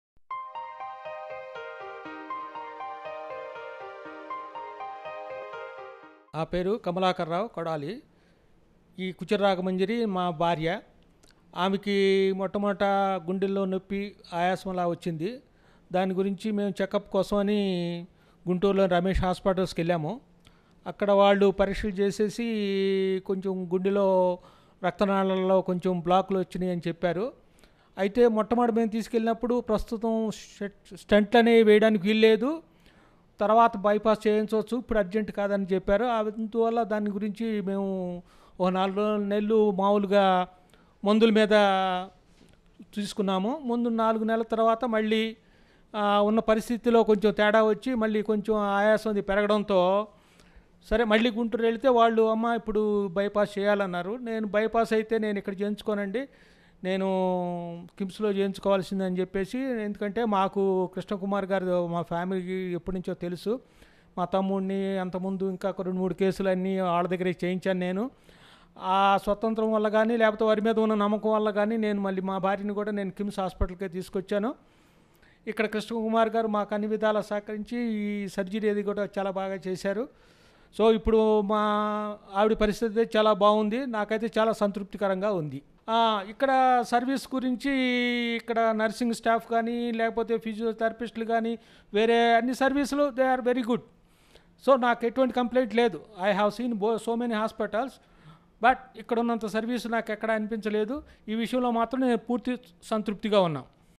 Coronary Bypass Surgery @ KIMS | Best Cardiologist in Hyderabad
Ms. Ragamanjari, 53 years old from Prakasam District was admitted for CABG (Coronary Artery Bypass Grafting) and was successfully treated by Dr. K. V. Krishna Kumar, Cardiothoracic and Cardiovascular Surgeon at KIMS Hospitals.